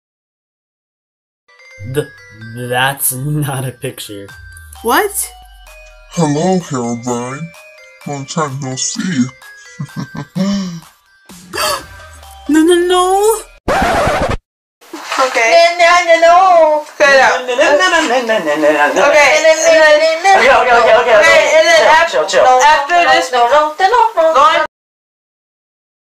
EAC2SNE: Enderman And Creeper Make Fun Of NULL
"This happened in the behind the scenes video for Enderman and Creeper 2 Short: NULL Encounter and I find it pretty funny. So I pretended that the characters acted the whole thing out."

Enderman and Creeper 2 Short: NULL Encounter

Enderman and Creeper 2 Short: NULL Encounter (BEHIND THE SCENES)

DaystormerMG27 YT's Channel
(Voice of Enderman)

Ozzy Wildeyes's Channel
(Voice of Creeper)

NULL28Ø2 YT's Channel
(Voice of NULL & Herobrine)